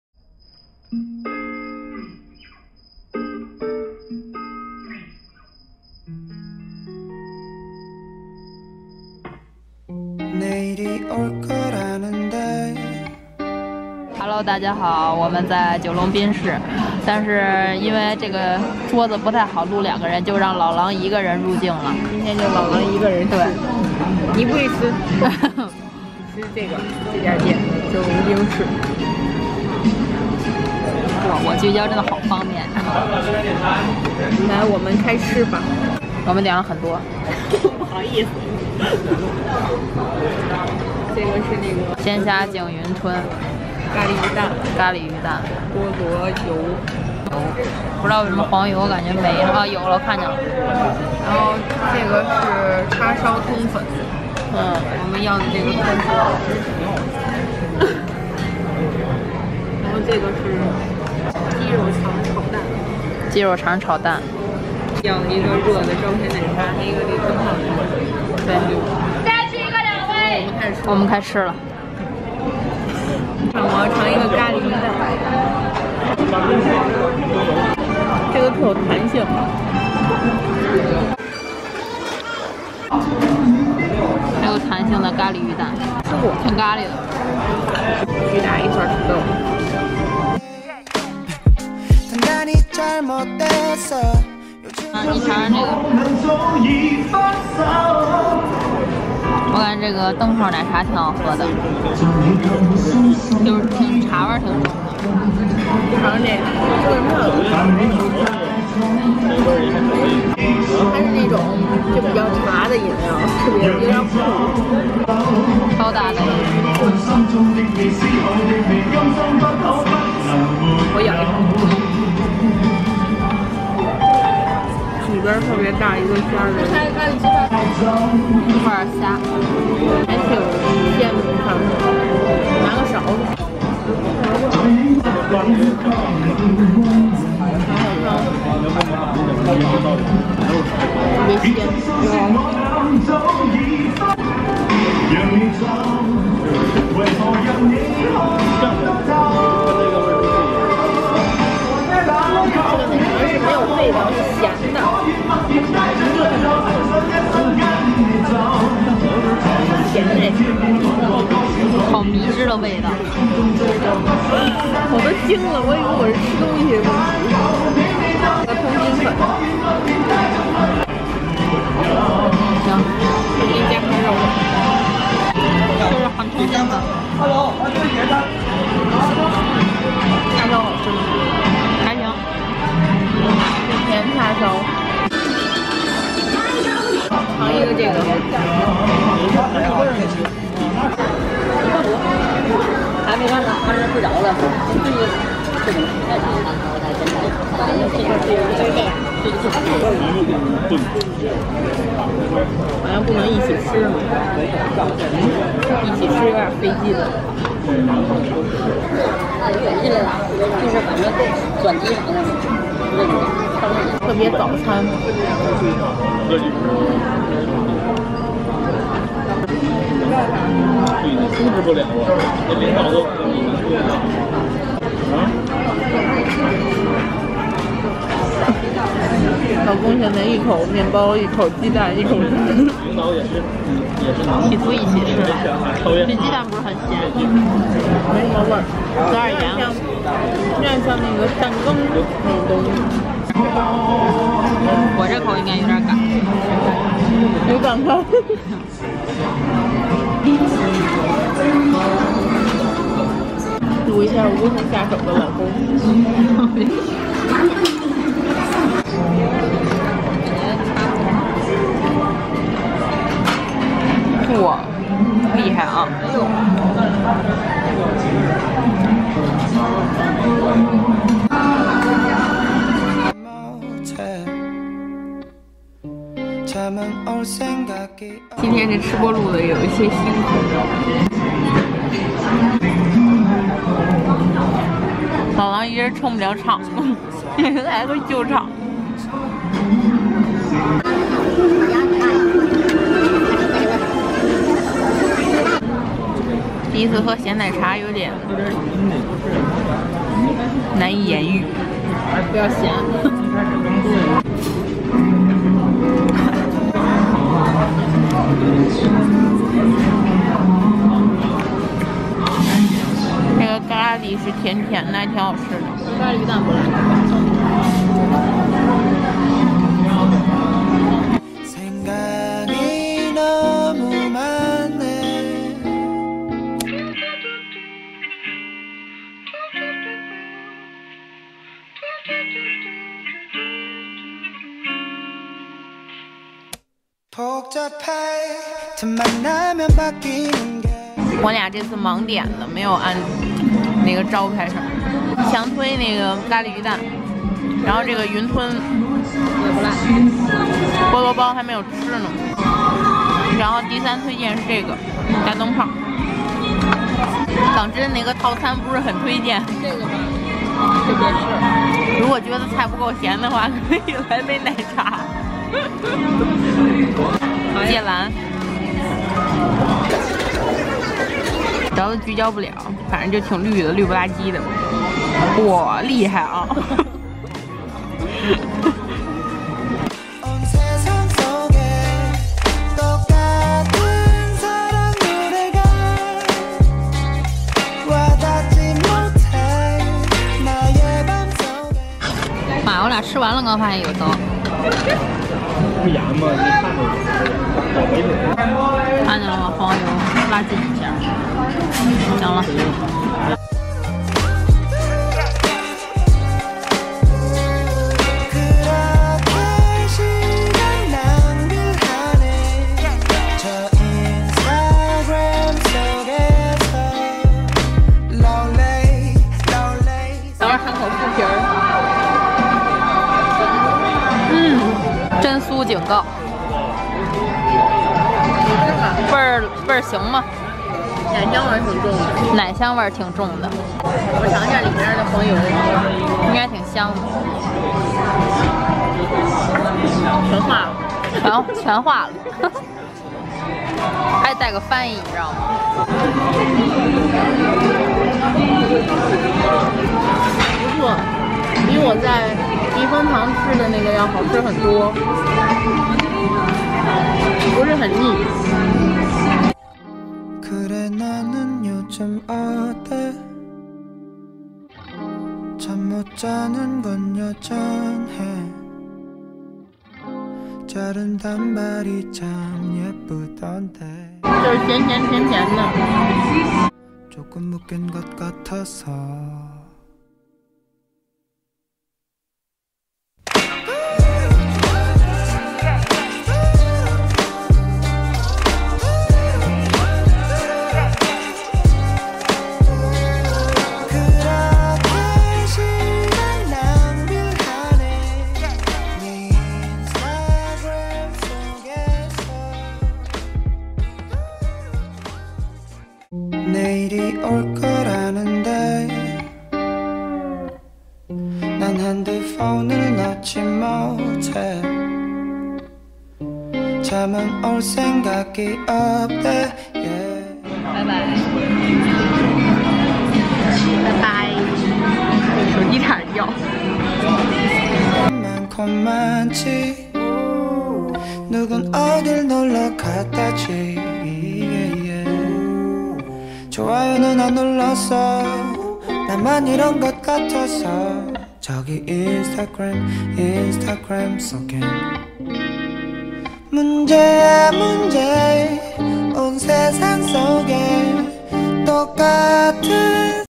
【探店】 北京三里屯网红港风餐厅 菠萝油 咖喱鱼蛋 叉烧通粉 鲜虾净云吞 香肠炒蛋 传统奶茶 适合拍照打卡
三里屯的九龙冰室 环境超港 拍照的好地方
咖喱鱼蛋 菠萝油 香肠炒蛋 叉烧通粉
鲜虾净云吞  奶茶